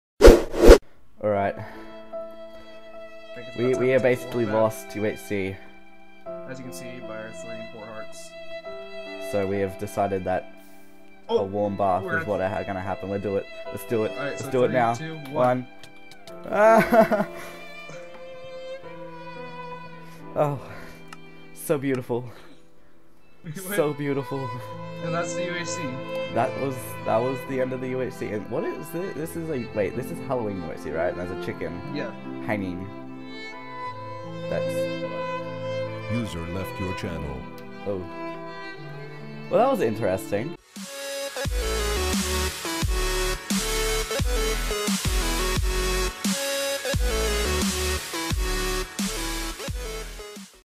The Poetic Death of Leo and Balls
We deaded in the recent synergize UHC! Hope You enjoy!

Goal: 200 Subscribers, 20 likes, 100 views :D Help me reach my goal